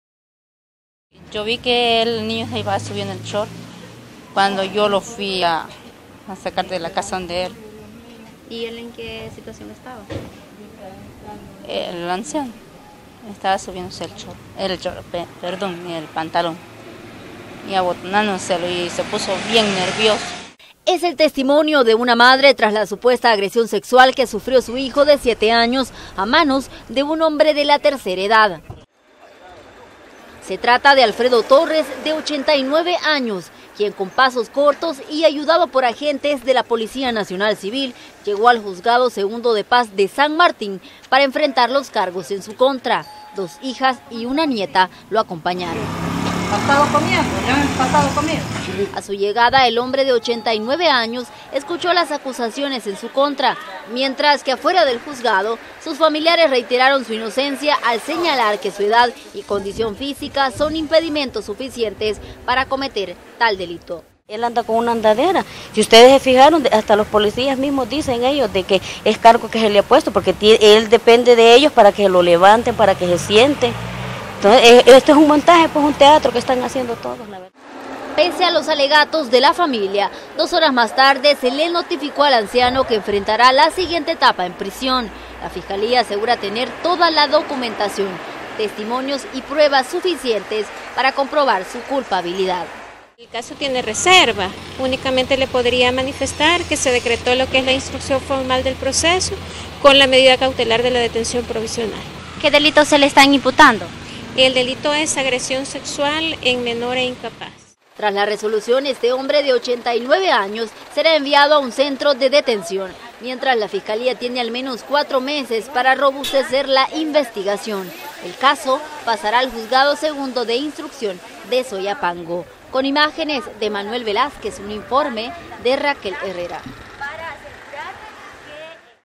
CN: A prisión hombre acusado de agresión sexual
A prisión fue enviado un hombre de 89 años acusado de agredir sexualmente a un niño de siete años.